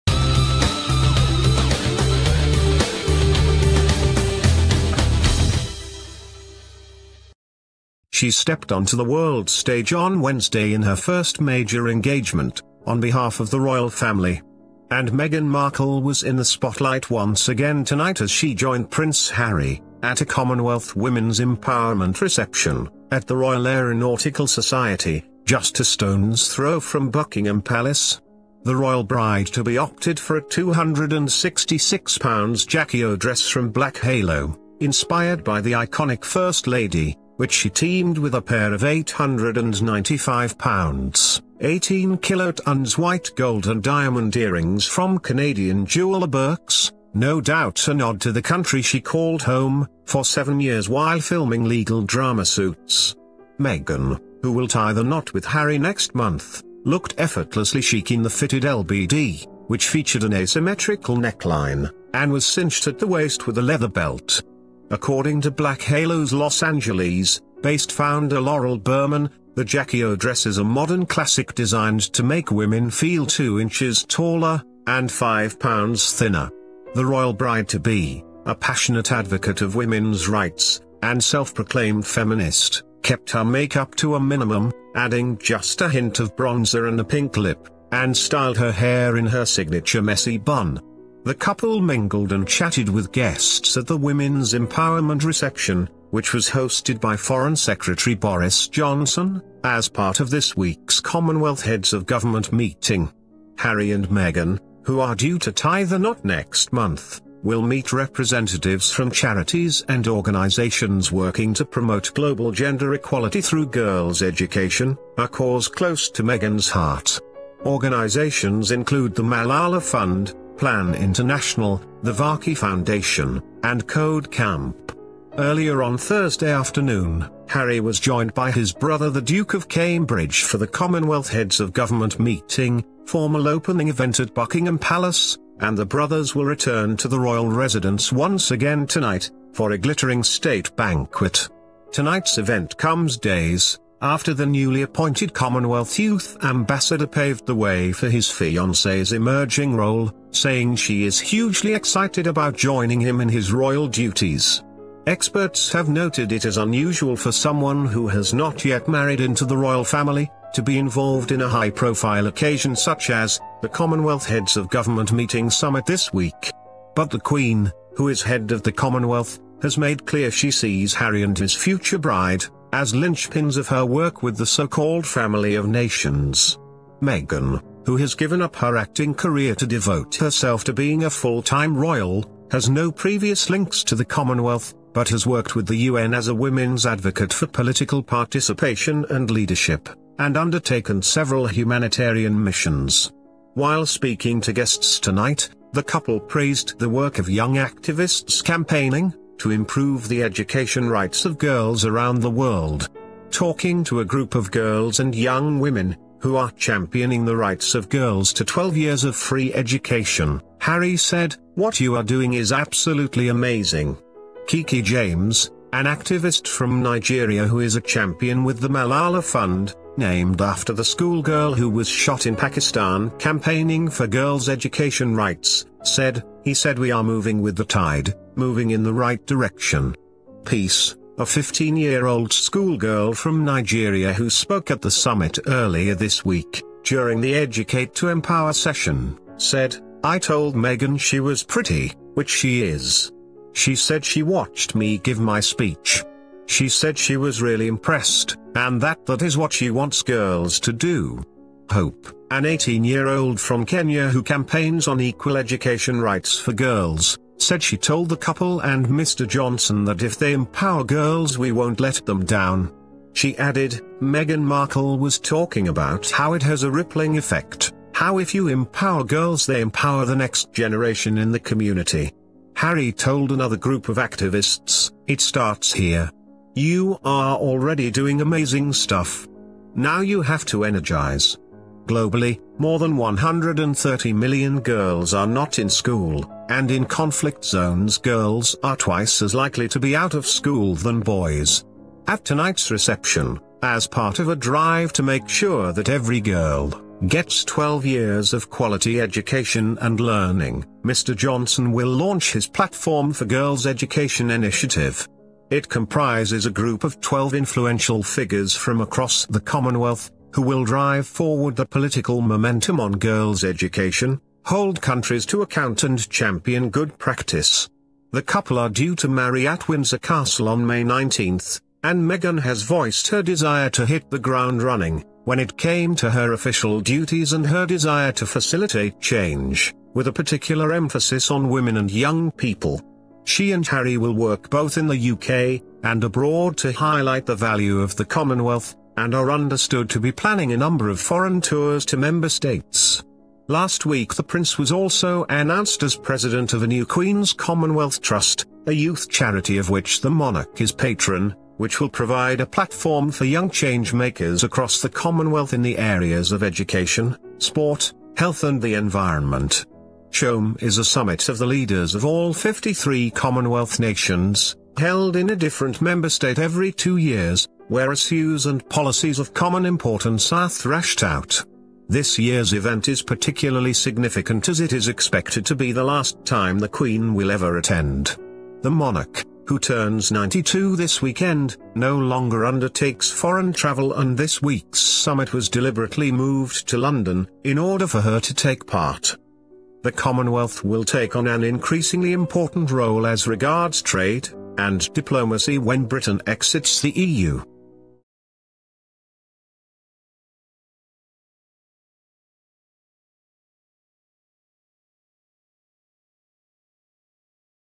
Meghan Markle and Prince Harry attended a CHOGM Women's Empowerment reception in London
The event, held at the Royal Aeronautical Society, was hosted by Foreign Secretary Boris Johnson.
This week marked Meghan's first major engagement on behalf of Royal family at Commonwealth reception.
Following the event, Prince Harry will return to Buckingham Palace for a state banquet hosted by the Queen.